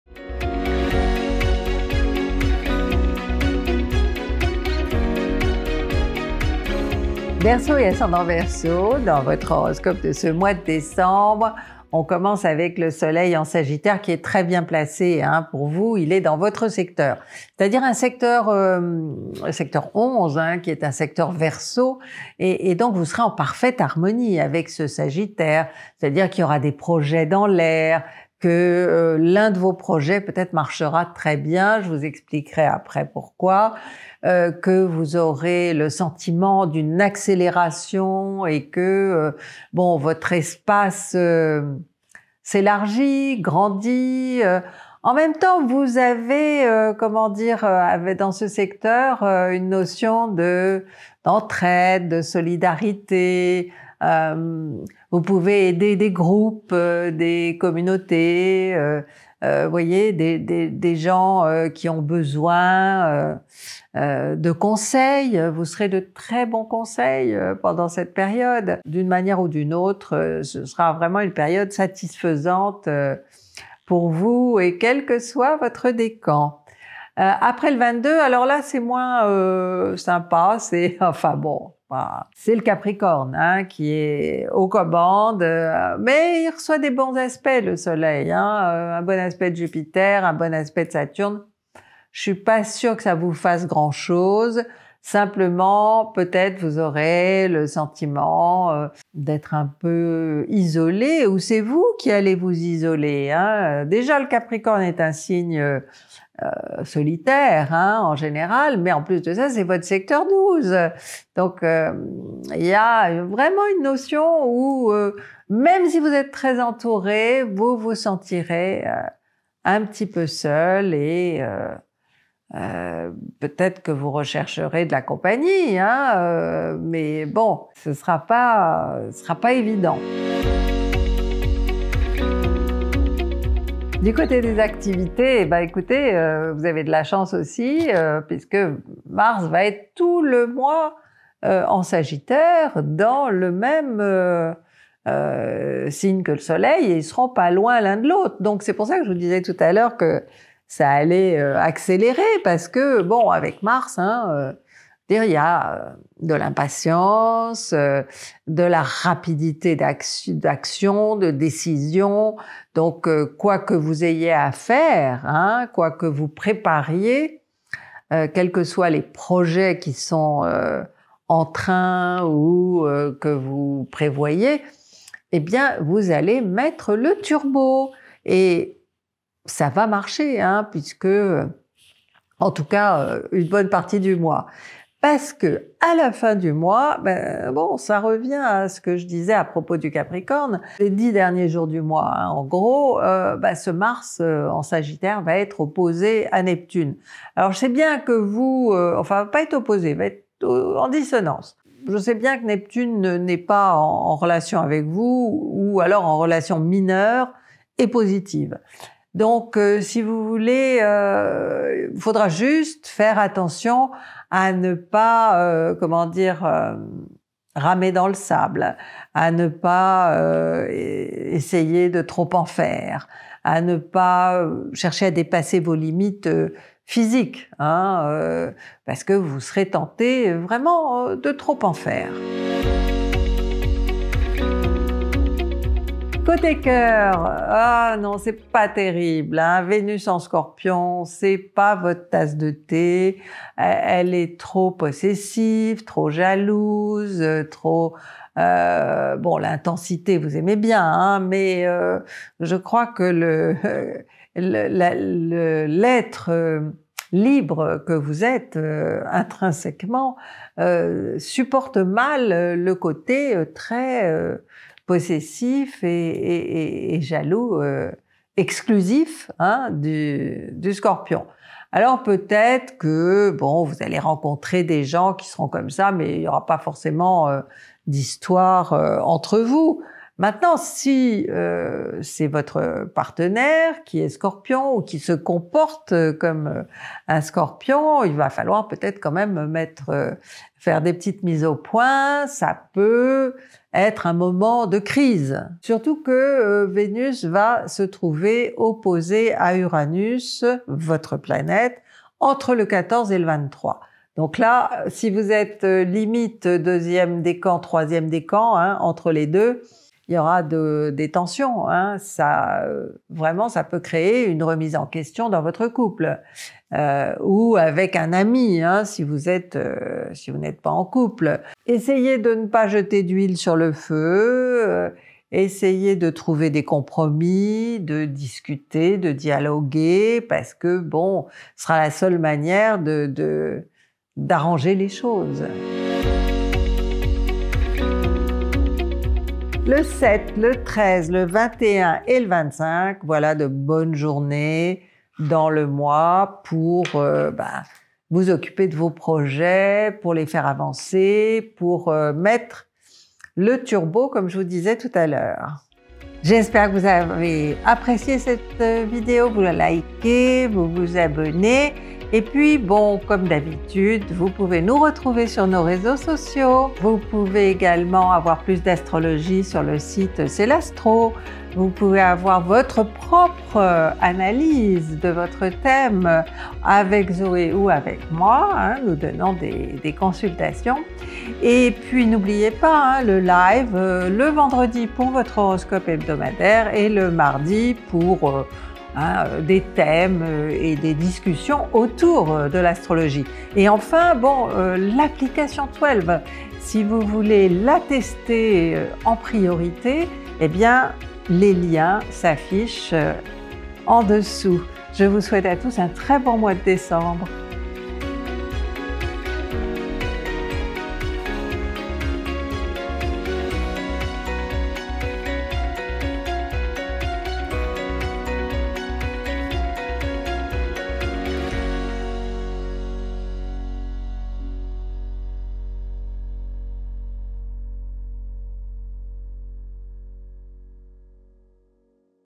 Horoscope Verseau ♒️ Décembre 2023 🎆 par Christine Haas
🌞 horoscope gratuit du signe du verseau pour le mois de decembre #2023 avec des conseils en astrologie et en psychologie.

⭐️ Retrouvez votre signe:
0:00 Introduction
0:06 Ambiance
2:10 Activités
3:52 Vie affective
5:35 Vos jours top
6:04 Bye bye